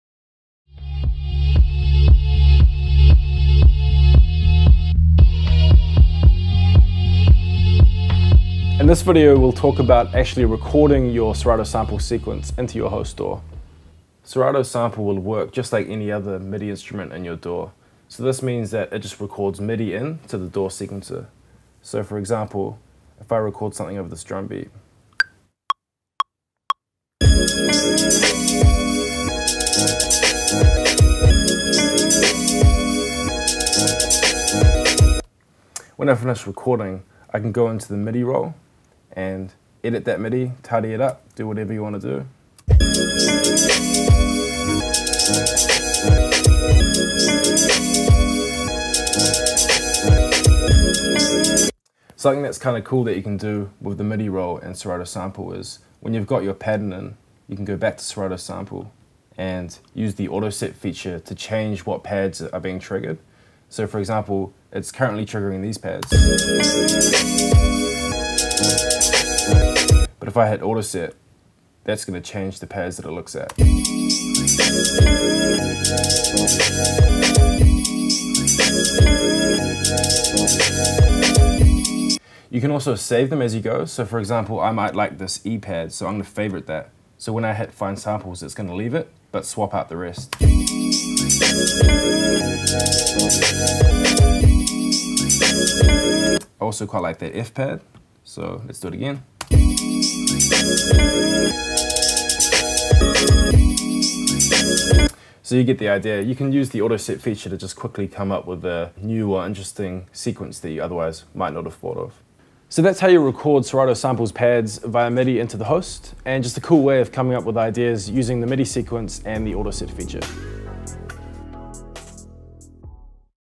Recording into your DAW with Serato Sample beta
Serato Sample works just like any other MIDI instrument, so it's super easy to record into your host DAW. Watch this tutorial to find out how.

Quickly discover, key shift & tempo stretch samples with this plugin from Serato.  All using the power of Pitch n’ Time.